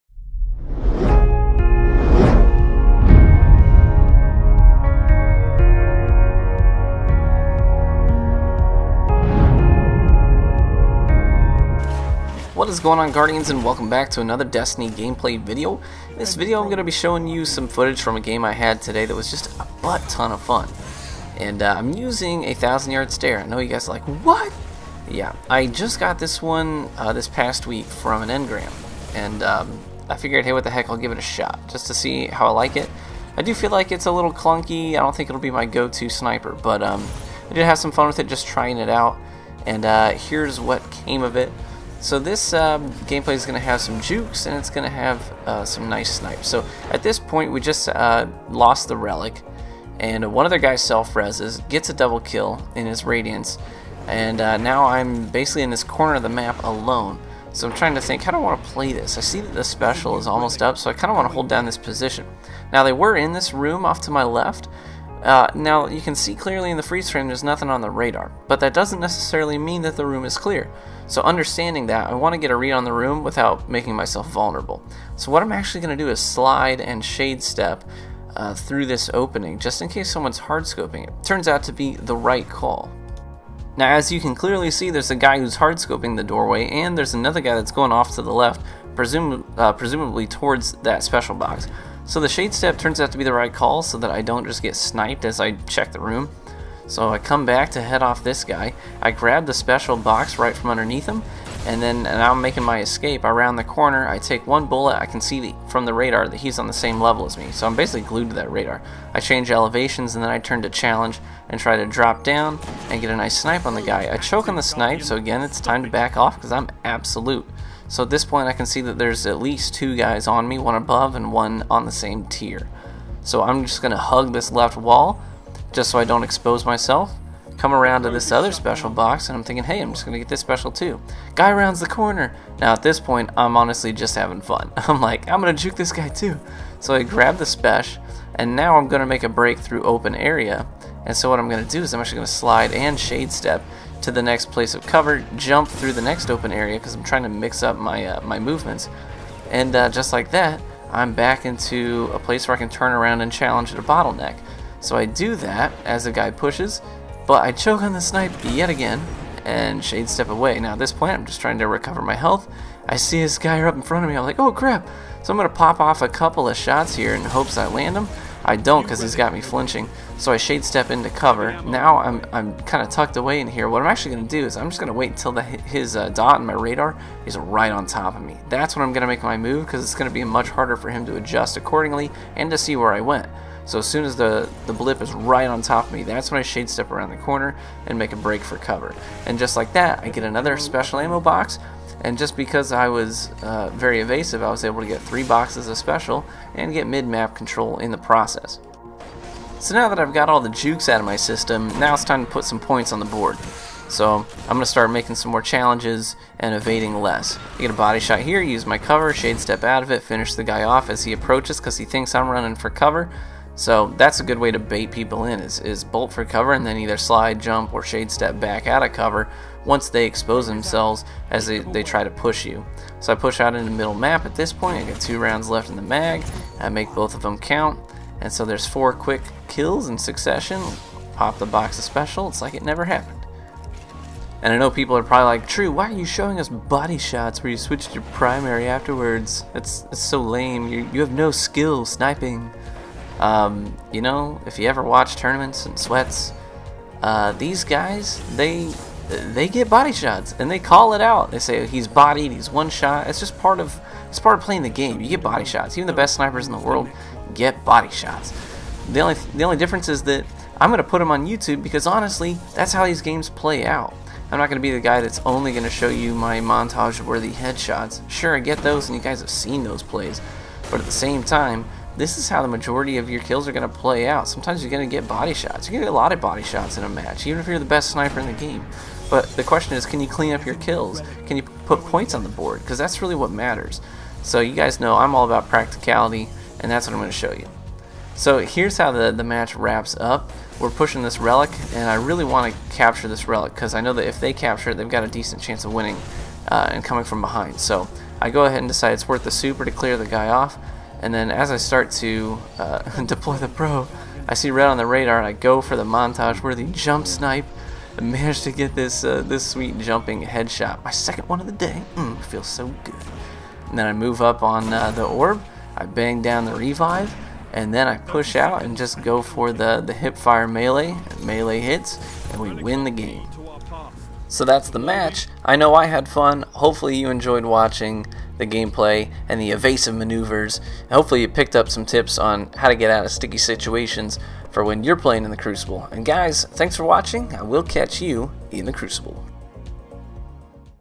Slippery Escapes and Sick Snipes with My New 1KS
My roll on the sniper is Shortgaze/Ambush, Quickdraw, Partial Refund, Spray and Play.  Not that amazing, but decent.